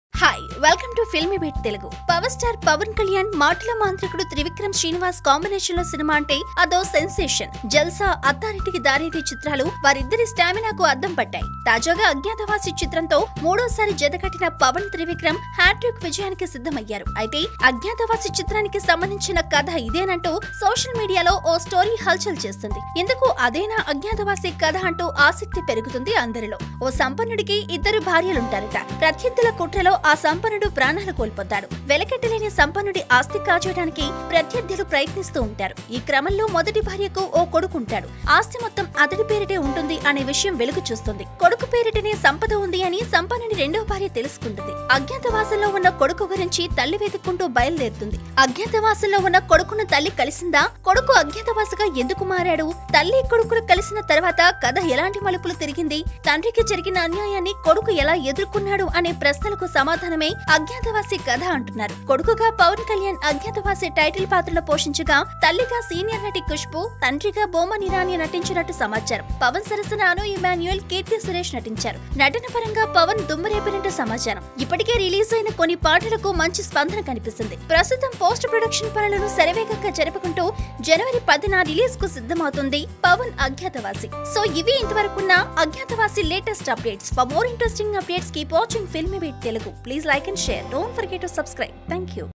అజ్ఞాతవాసి కథ ఇదేనట..! | Filmibeat Telugu
Pawan Kalyan's Agnathavasi story viral in social media. This movie story is quiet interesting. This movie set to release on January 2018. Meanwhile, huge preparatons are for Agnathavasi audio is on card.

పవర్‌స్టార్ పవన్ కల్యాణ్, మాటల మాంత్రికుడు త్రివిక్రమ్ శ్రీనివాస్ కాంబినేషన్‌లో సినిమా అంటే అదో సెన్సేషన్. జల్సా, అత్తారింటికి దారేది చిత్రాలు వారిద్దరి స్టామినాకు అద్దం పట్టాయి. తాజాగా అజ్ఞాతవాసి చిత్రంతో మూడోసారి జతకట్టిన పవన్, త్రివిక్రమ్ హ్యాట్రిక్ విజయానికి సిద్ధమవయ్యారు. అయితే అజ్ఞాతవాసి చిత్రానికి సంబంధించిన కథ ఇదేనంటూ..సోషల్ మీడియాలో ఓ స్టోరి హల్‌చల్ చేస్తున్నది. ఇంతకు అదేనా అజ్ఞాతవాసి కథ అంటూ ఆసక్తి పెరుగుతున్నది.
ఓ సంపన్నుడికి ఇద్దరు భార్యలు ఉంటారు. ప్రత్యర్థుల కుట్రలో ఆ సంపన్నుడు ప్రాణాలు కోల్పోతాడు. వెలకట్టలేని సంపన్నుడి ఆస్తి కాజేయడానికి ప్రత్యర్థులు ప్రయత్నిస్తుంటారు. ఈ క్రమంలో మొదటి భార్యకు ఓ కొడుకు ఉంటాడు. ఆస్తి మొత్తం అతడి పేరిటే ఉంటుంది అనే విషయం వెలుగు చూస్తుంది.
కొడుకు పేరిటనే సంపద ఉంది అని సంపన్నుడి రెండో భార్య తెలుసుకొంటుంది. అజ్ఞాతవాసంలో ఉన్న కొడుకు గురించి తల్లి వెతుక్కుంటూ బయలుదేరుతుంది. అజ్ఞాతవాసంలో ఉన్న కొడుకును తల్లి కలిసిందా? కొడుకు అజ్ఞాతవాసిగా ఎందుకు మారాడు. తల్లి, కొడుకులు కలిసిన తర్వాత కథ ఎలాంటి మలుపులు తిరిగింది. తండ్రికి జరిగిన అన్యాయాన్ని కొడుకు ఎలా ఎదుర్కొన్నాడు అనే ప్రశ్నలకు సమాధానమే అజ్ఞాతవాసి కథ అంటున్నారు.